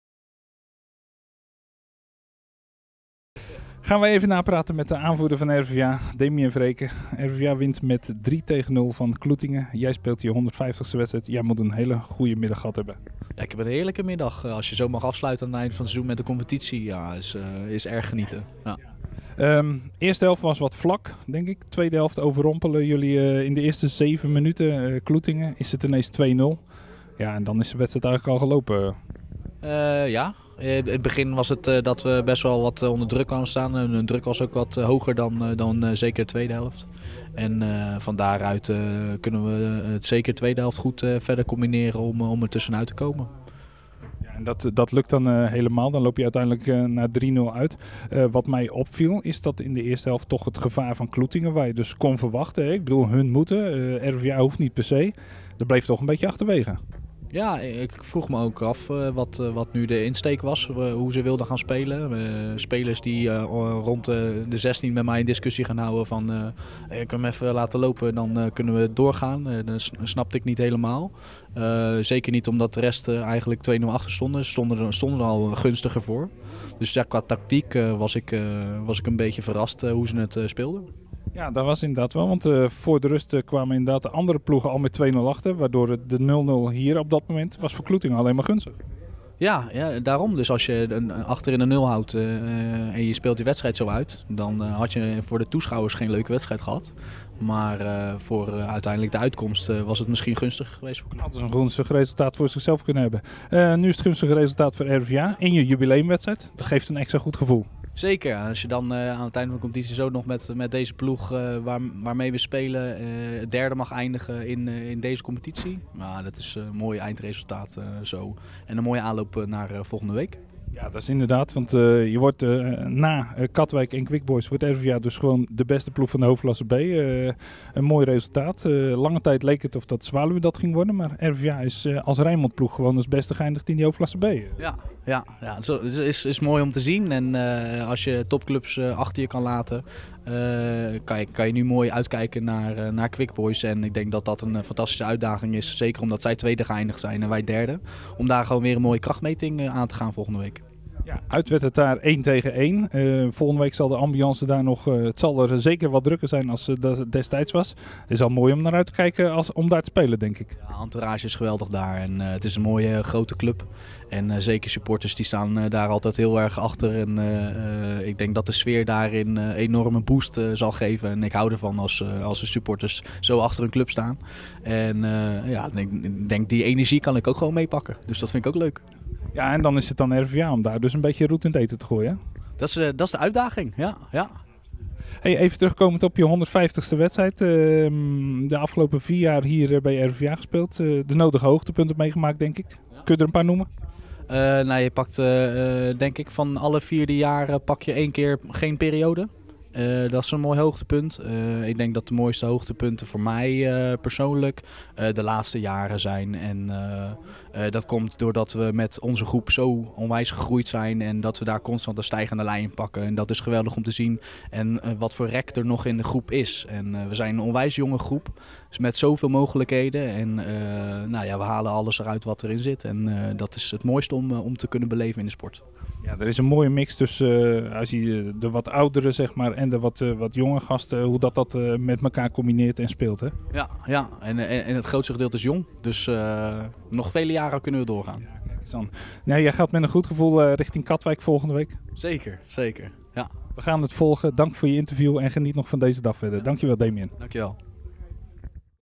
RVVH - Kloetinge Interview Damien Vereecken
Het interview met Damien Vereecken, die op 9 mei 2015 zijn 150ste wedstrijd voor RVVH speelde, na afloop van RVVH - Kloetinge in de Hoofdklasse B.